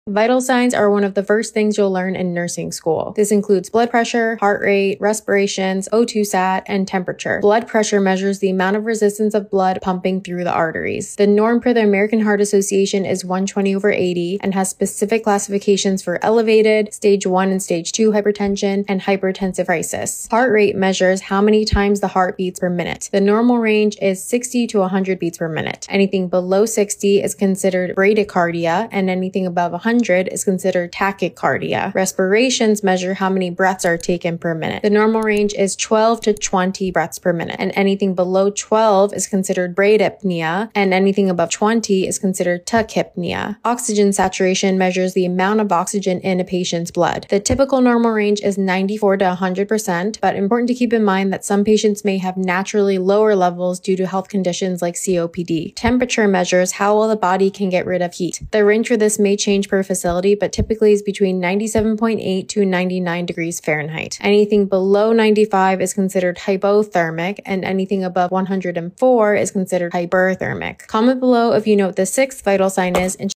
Vital Signs in 60 Seconds! 🩺
🩺 Description

Learn the 5 vital signs every nurse must know! 💉
In this short video, you’ll quickly understand what vital signs are, how they’re measured, and why they’re essential in patient care.
Perfect for nursing students, beginners, and healthcare professionals who want a fast refresher. ⏱️

scrub talks

scrub talks channel